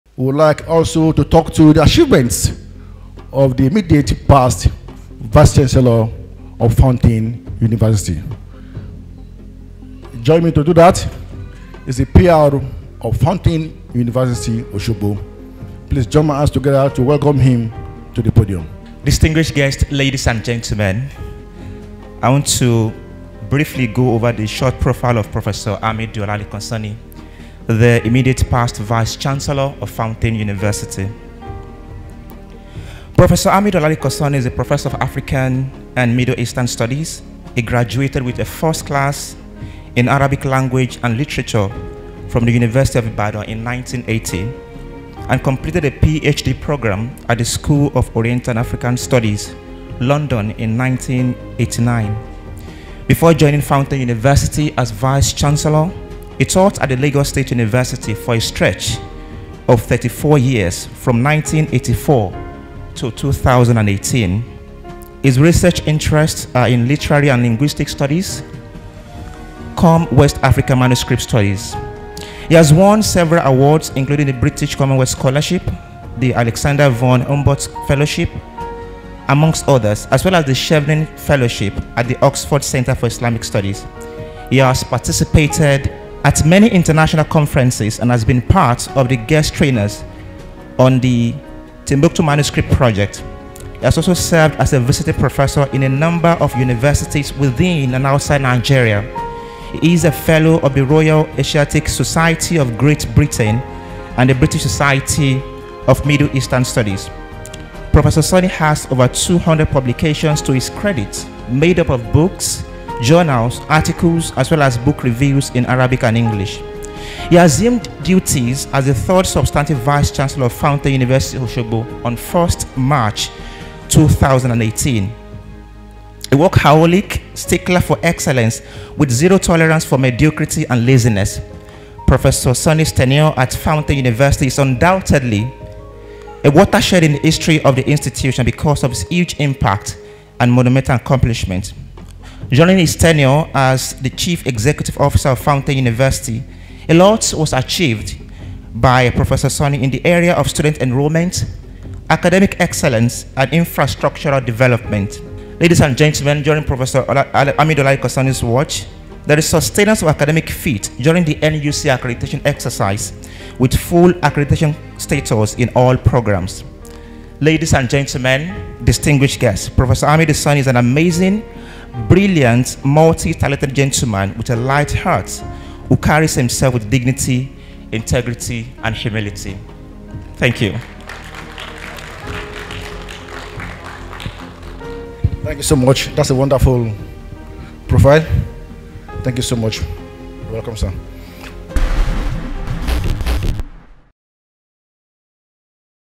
Short profile of Prof. Amidu Olalekan Sanni, the 3rd Substantive VC of Fountain University, Osogbo.
Though I was called to deliver this profile with zero prior knowledge that I'd be doing that, it will always be an honour that I handled this part of the programme.

Cheers to a respected and brilliant Chief Executive Officer I worked with at @fountainUni2013 , Prof. Amidu Olalekan Sanni.

By the way, we should talk about me handling your next corporate event.